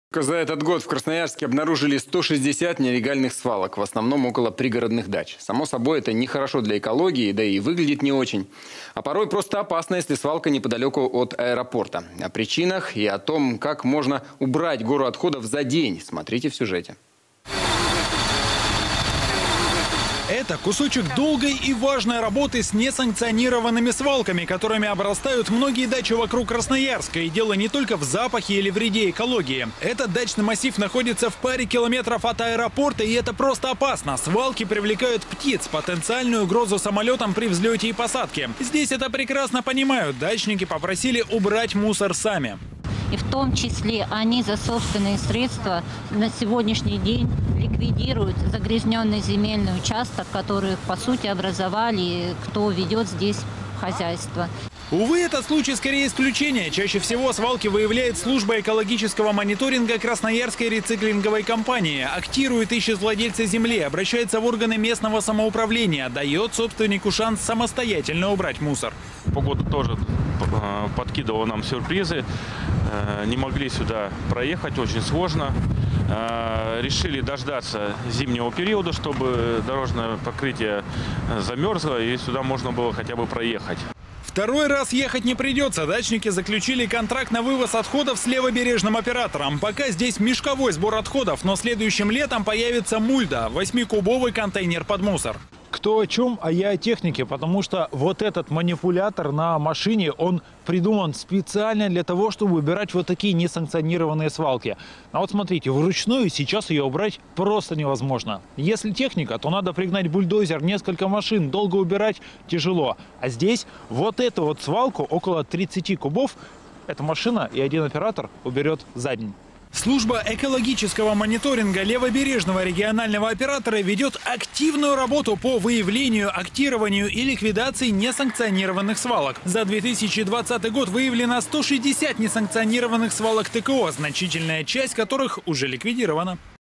За год в городе обнаружили 160 нелегальных свалок
Только за этот год в Красноярске обнаружили 160 нелегальных свалок, в основном около пригородных дач. Само собой, это нехорошо для экологии, да и выглядит не очень. А порой просто опасно, если свалка неподалёку от аэропорта. О причинах и том, как можно убрать гору отходов за день смотрите в сюжете.

Это кусочек долгой и важной борьбы с несанкционированными свалками, которыми обрастают многие дачи вокруг Красноярска. И дело не только в запахе и вреде экологии. Этот дачный массив находится в паре километров от аэропорта это просто опасно, свалки привлекают птиц — потенциальную угрозу самолетам при взлете и посадке. Здесь это прекрасно понимают, дачники попросили убрать мусор сами.

Наталья Орлова, руководитель отдела экологического мониторинга «Красноярской рециклинговой компании»:
— И в том числе они, за собственные средства, ликвидируют загрязнённый земельный участок, который по сути образовали те, кто ведёт здесь хозяйство.

Увы, этот случай скорее исключение. Чаще всего свалки выявляет служба экологического мониторинга «Красноярской Рециклинговой компании», актирует, ищет владельца земли, обращается в органы местного самоуправления, дает собственнику шанс самостоятельно убрать мусор.

Святослав Шалимов, заместитель директора обособленного подразделения ООО «Экопроект»:
— Мы пытались данную свалку ликвидировать ещё в летний период, но погода тоже подкидывала нам сюрпризы: не могли сюда проехать, очень сложно. Решили дождаться зимнего периода, чтобы дорожное покрытие замёрзло и сюда можно было хотя бы проехать.

Второй раз ехать не придётся — дачники заключили контракт на вывоз отходов с левобережным оператором. Пока здесь был мешковой сбор отходов, но следующим летом появится мульда — 8 кубовый контейнер под мусор.

Кто о чём, а я о технике. Вот этот манипулятор на машине придуман специально для того, чтобы убирать такие несанкционированные свалки. Смотрите, вручную сейчас её убрать просто невозможно. Если техника, то надо пригнать бульдозер, несколько машин, долго убирать, тяжело. А здесь эту свалку, около 30 кубов, эта машина и один оператор уберёт за день.

Служба экологического мониторинга левобережного регионального оператора ведет активную работу по выявлению, актированию и ликвидации несанкционированных свалок. За 2020 год выявлено — 160 несанкционированных свалок ТКО, значительная часть которых уже ликвидирована.